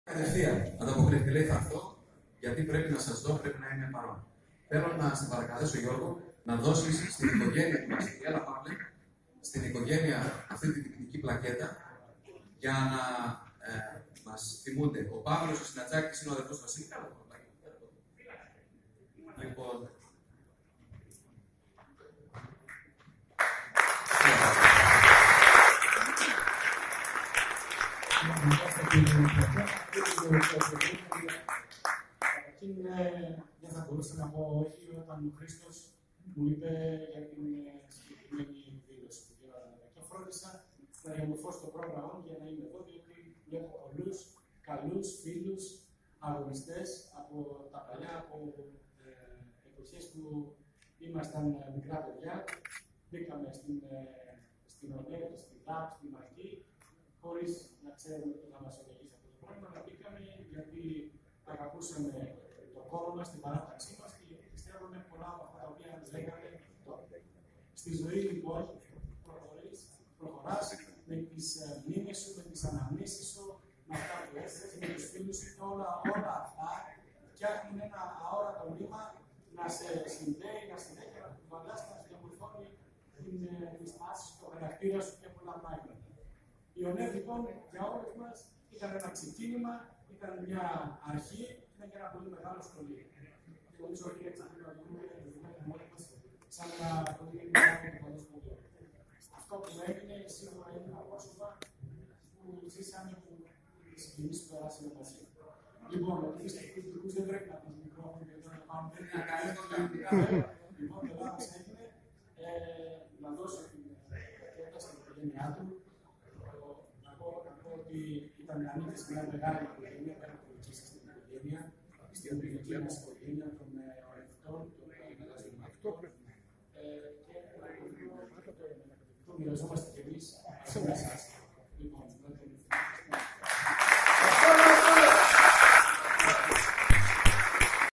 Εκδήλωση ΟΝΝΕΔ Θεσσαλονίκης δεκαετίας ΄80 στην μνήμη του Βασίλη Συναντσάκη - 3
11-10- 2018 - Κέντρο Πολιτισμού ΠΚΜ'' Αλέξανδρος''   -Εκδήλωση μελών και φίλων της ΟΝΝΕΔ -  Θεσσαλονίκης την δεκαετία του ΄80 στην μνήμη του συνθέτη του συγκροτήματος της νεολαίας της Ν.Δ. Βασίλη Συναντσάκη με τίτλο '' Γαλάζιοι Μαχητές-  Ο βουλευτής Άρτας της Ν.Δ. Γιώργος Στύλιος Οννεδίτης και Δαπίτης την δεκαετία του ΄80 ,απευθύνει χαιρετισμό στην εκδήλωση,απονέμει τιμητική πλακέτα στον αδερφό του Βασίλη Συναντσάκη Πάυλο,κάτω από τα ενθουσιώδη χειροκροτήματα των παραρευρισκόμενων .